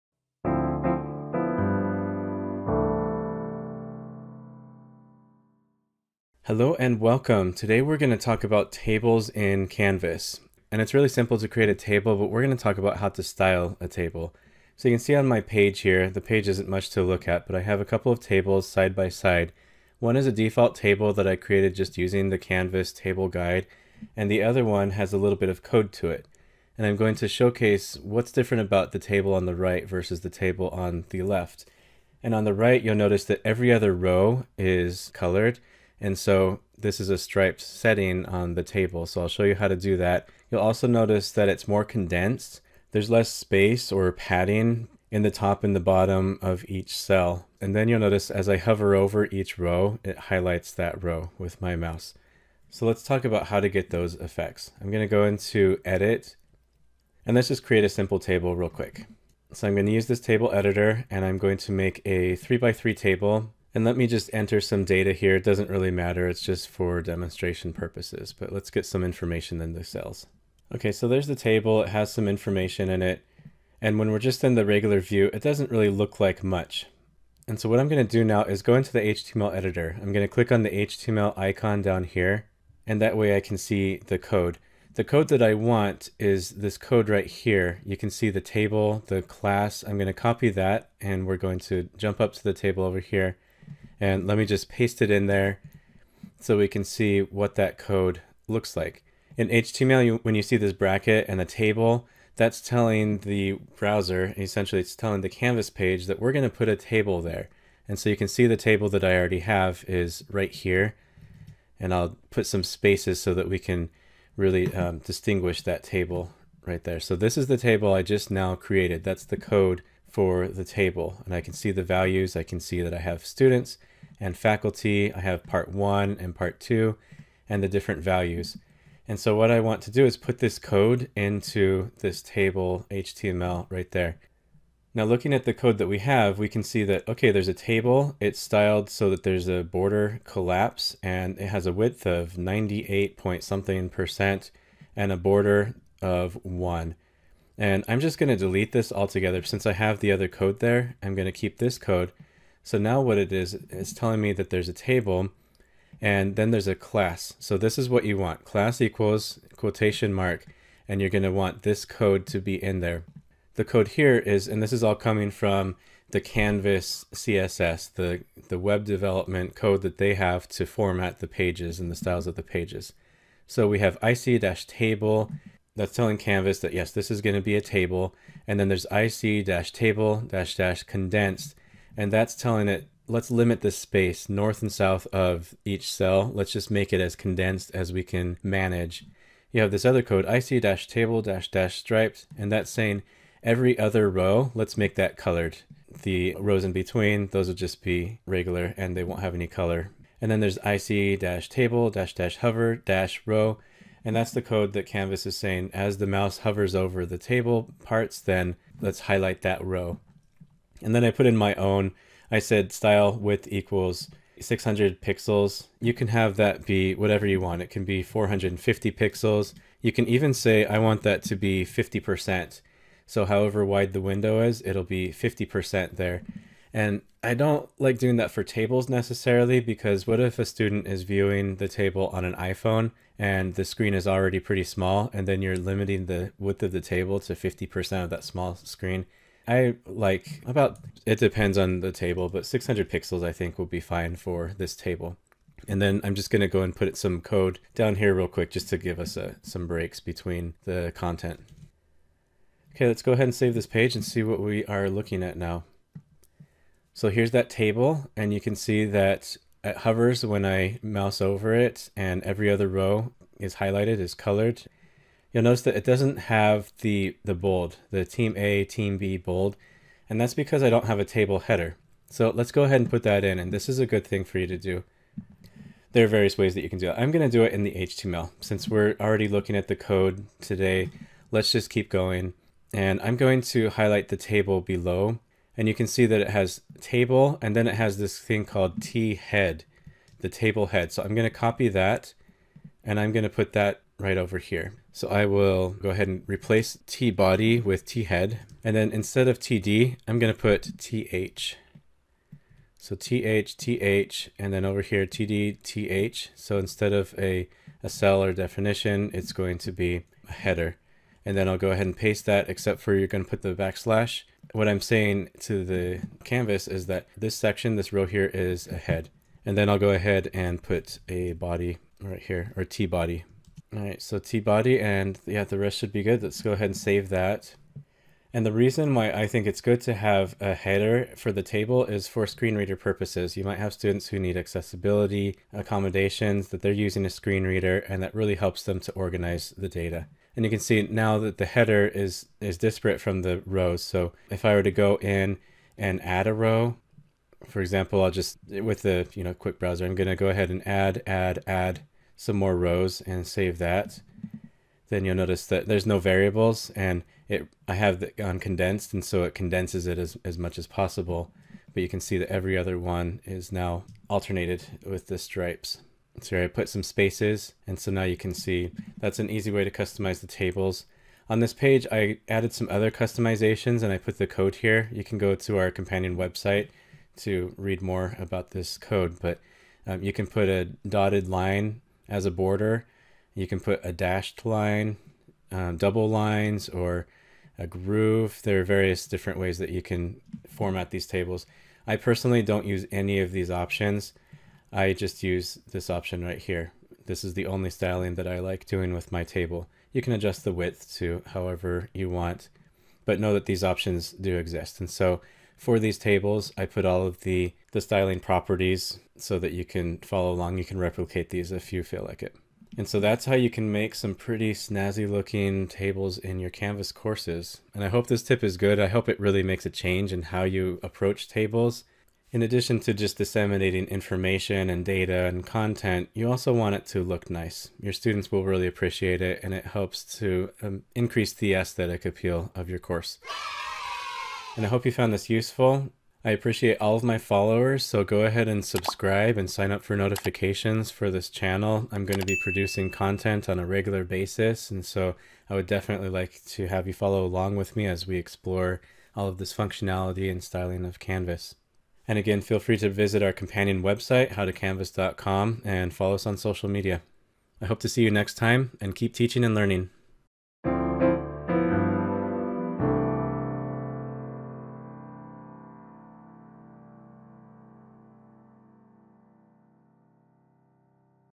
Styling Tables in Canvas
Table are inherently boring, but you can learn how to easily apply some amazing styling with just a little bit of coding.  Dress up your tables in your Canvas courses and become the envy of your colleagues.

To create the interactive table, here is the code you will want to copy:

table class="ic-Table ic-Table--condensed ic-Table--striped ic-Table--hover-row" style="width: 600px;"
(remember to include the angled brackets, which YouTube doesn't allow in these descriptions)

Read